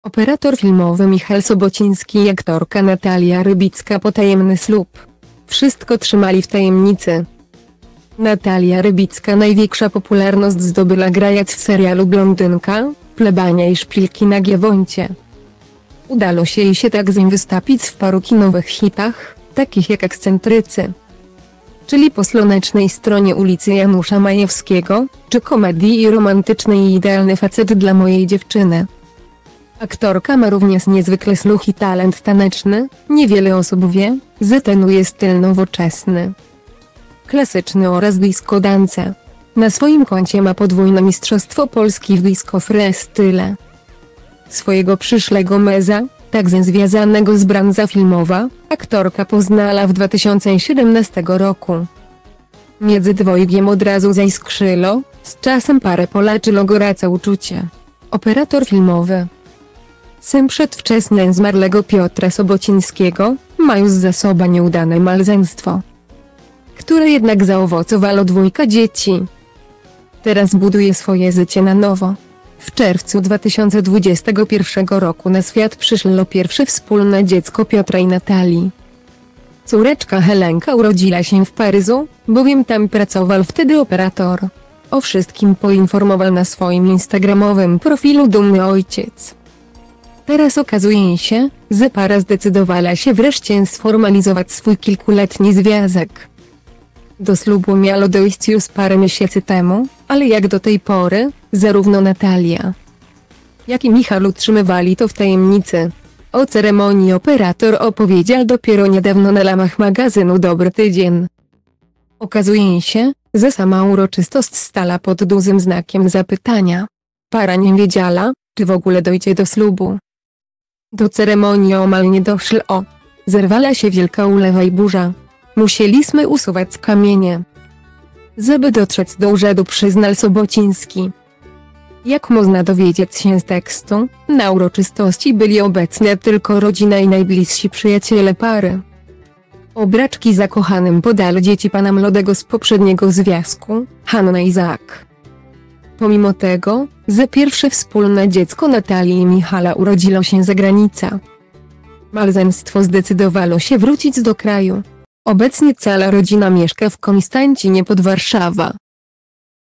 Operator filmowy Michal Sobocinski i aktorka Natalia Rybicka potajemny slub! Wszystko trzymali w ta
Operator filmowy Michal Sobocinski i aktorka Natalia Rybicka  potajemny slub! Wszystko trzymali w tajemnicy
Natalia Rybicka najwieksza popularnosc zdobyla grajac w serialu "Blondynka", "Plebania" i "Szpilki na Giewoncie".Udalo sie jej sie takze wystapic w paru kinowych hitach, takich jak "Excentrycy,czyli po slonecznej stronie ulicy" Janusza Majewskiego, czy komedii romantycznej "Idealny facet dla mojej dziewczyny".